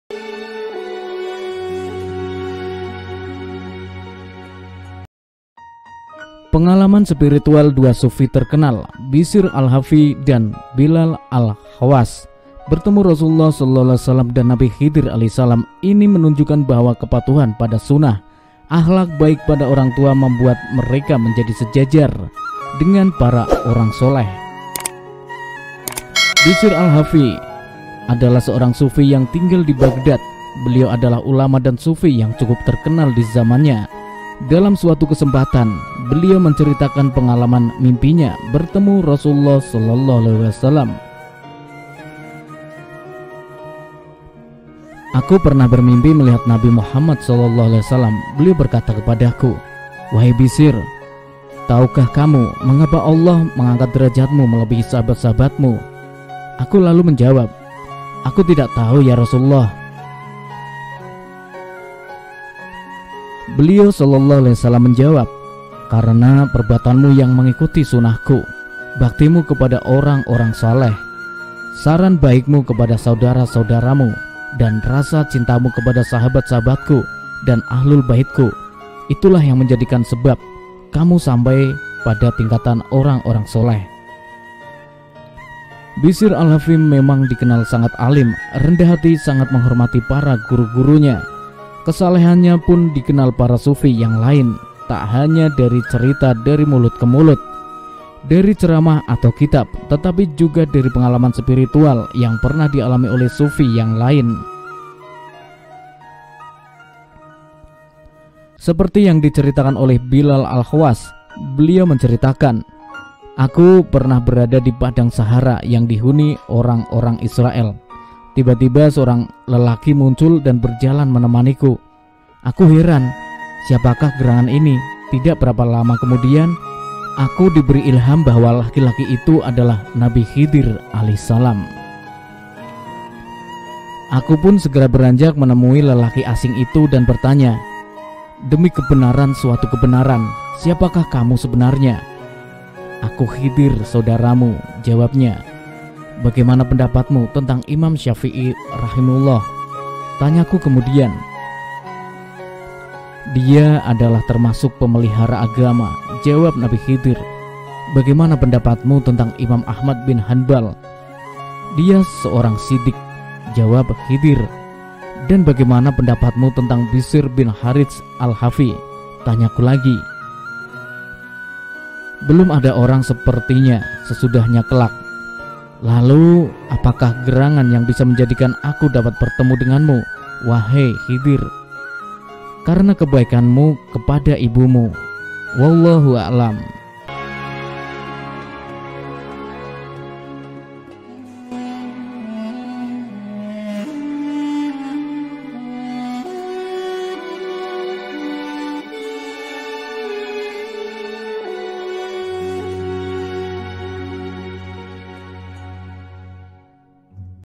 Kisah Hikmah: Karena Baktinya Pada Ibunya, Siapa Sangka Pria Ini Bertemu Nabi Khidir AS.. MasyaAllah
BisyrAl-Hafi